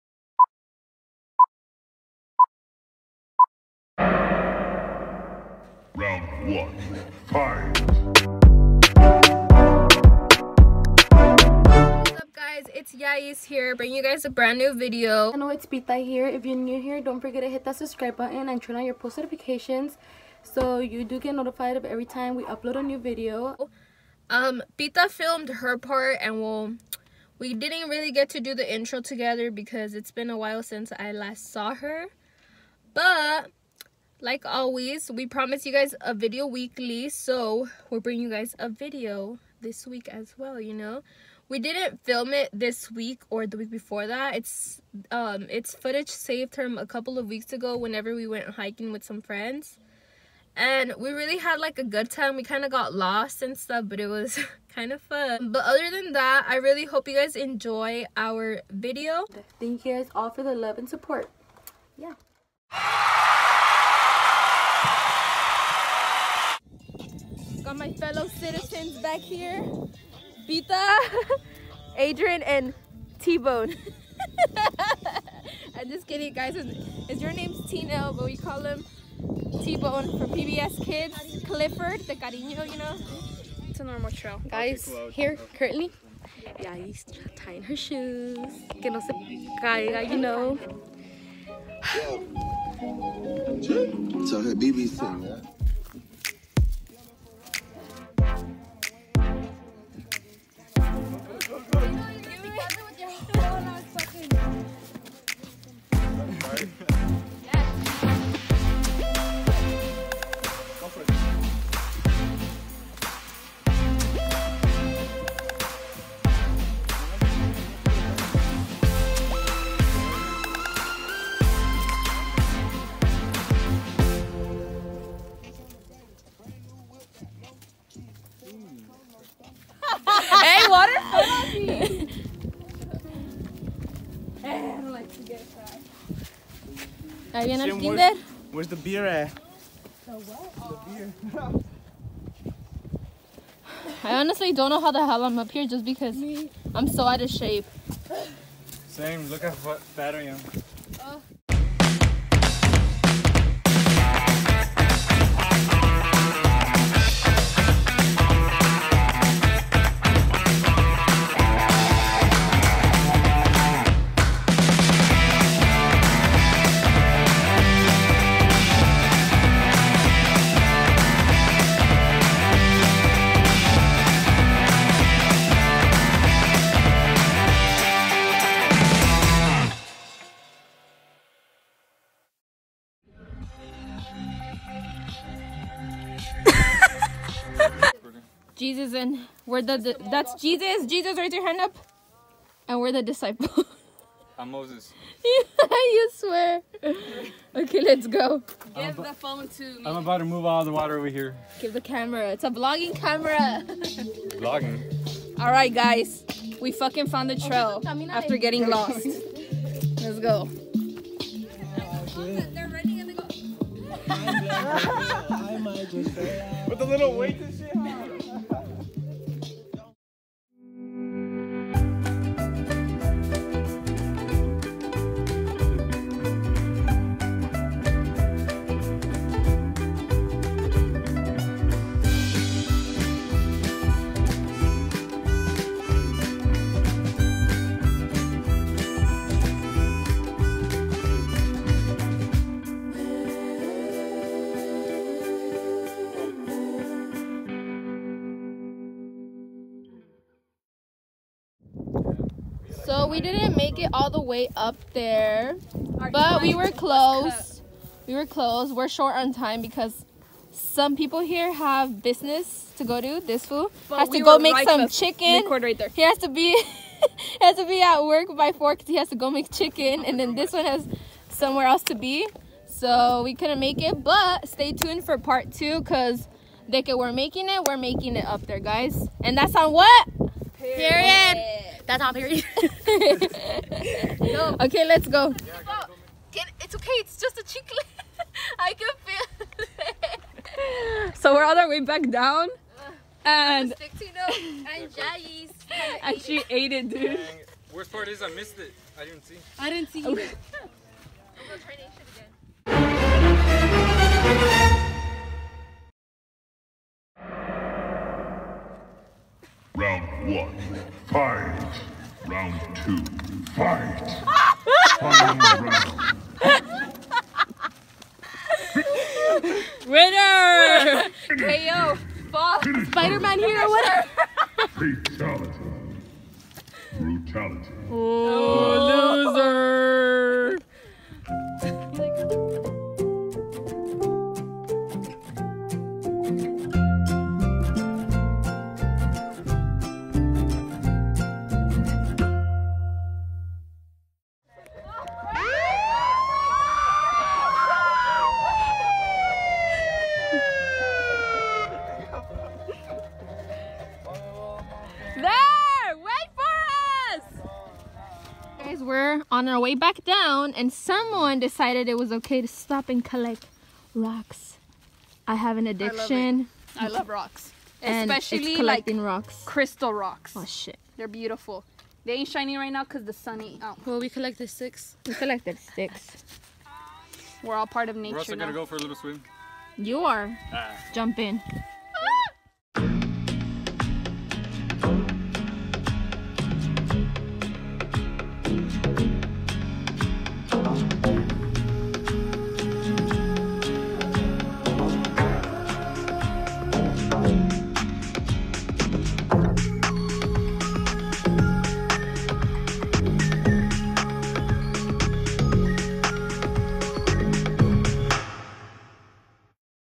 HIKING W FRIENDS ⛰
HELLO LOVES, we are back with another video. We hope you all enjoy this short mini vlog of us hiking with some friends. The hike its self was really fun.

 THANK YOU SOOOOO MUCH FOR THE LOVE AND SUPPORT!! 💗